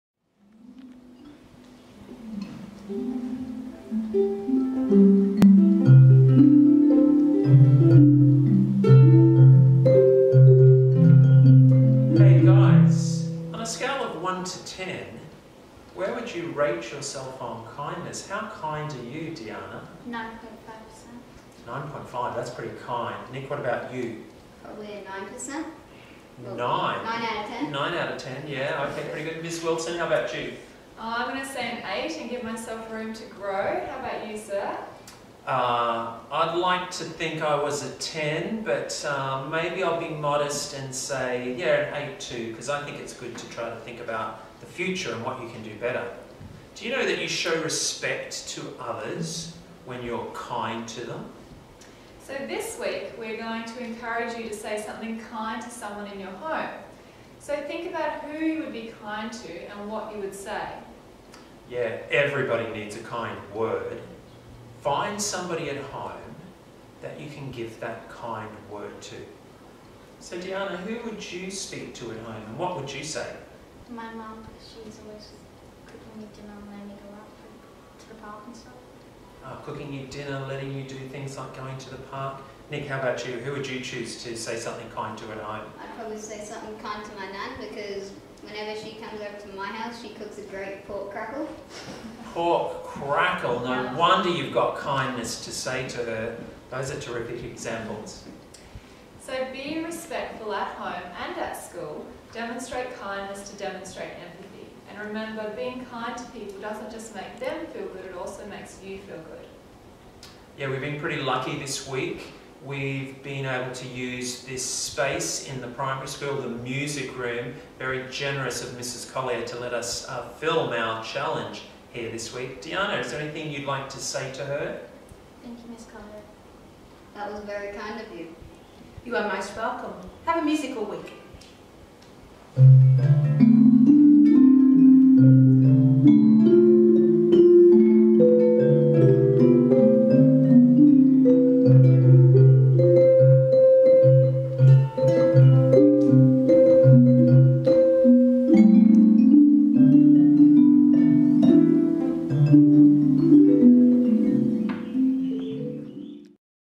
The Resilience Challenge - T4W7 Empathy
The Resilience Project focus for Term 4 Week 7 is EMPATHY.
During Term 4, we will continue to present a Resilience Challenge each week, which students view in class. To remain connected, parents/carers and community members can also view these presentations on the College's YouTube channel and Facebook page.